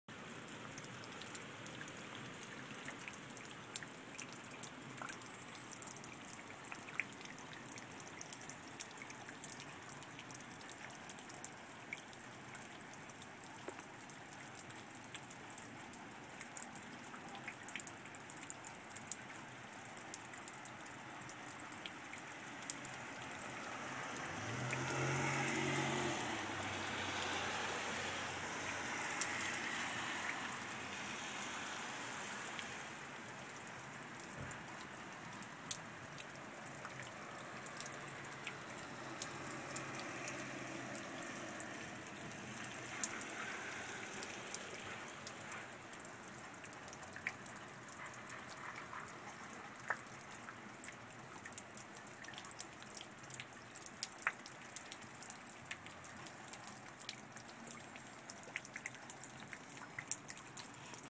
Double Rainbow iPhone footage
Video of a double rainbow from my front yard.